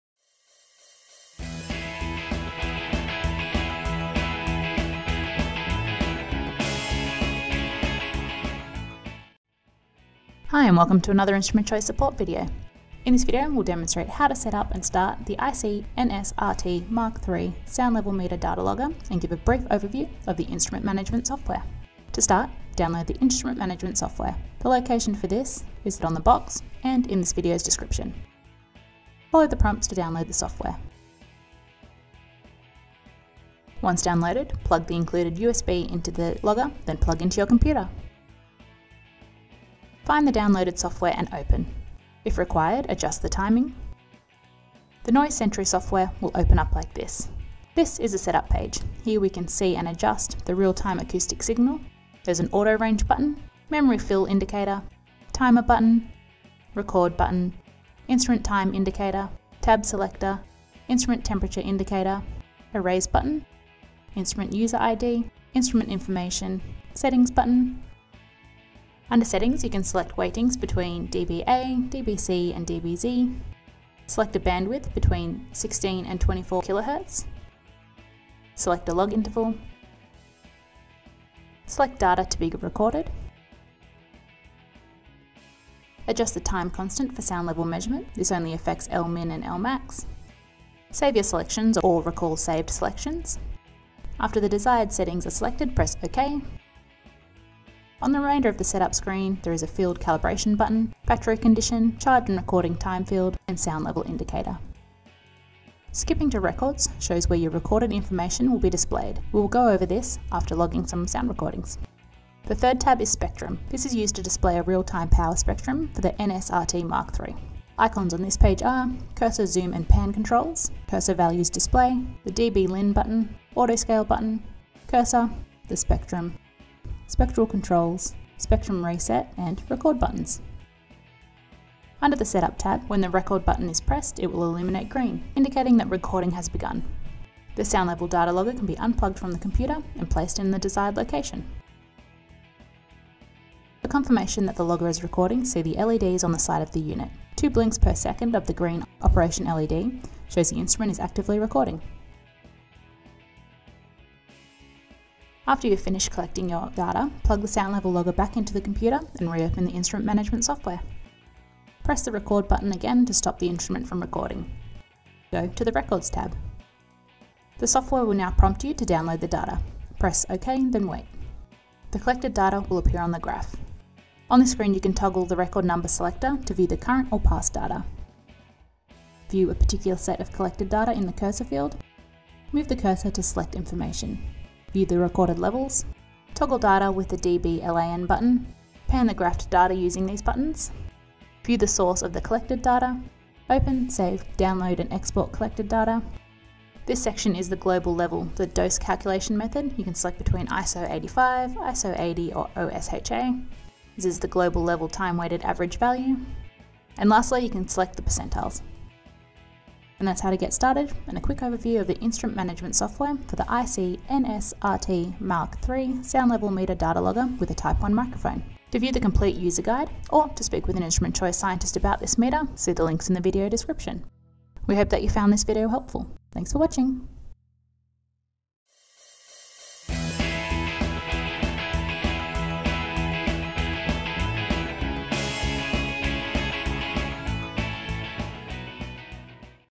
How to Set up and Start the IC-NSRT_mk3 Sound Level Meter Data Logger with Type 1 Microphone
In this video, an Instrument Choice Scientist demonstrates how to set up, start and view collected data on the IC-NSRT_mk3 Sound Level Meter Data Logger with Type 1 Microphone.